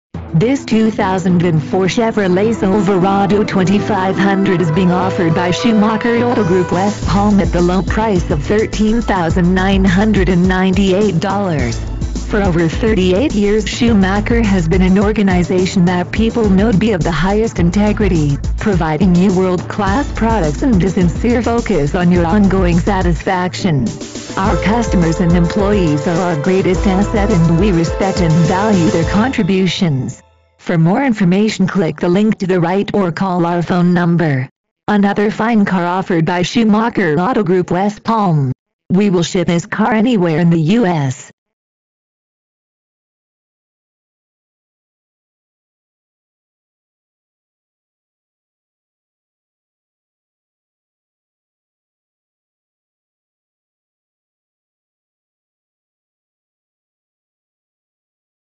2004 CHEVROLET SILVERADO 2500 west palm beach FL
Price: $ 13,998
Mileage: 83,994
Body Style: XCAB
Ext. Color: GREEN
Int. Color: GRAY
Engine: Vortec OHV V8
Drive Type:
Fuel Type:
Doors:
Stock No.: G190071B
VIN: 1GCHK29UX4E222357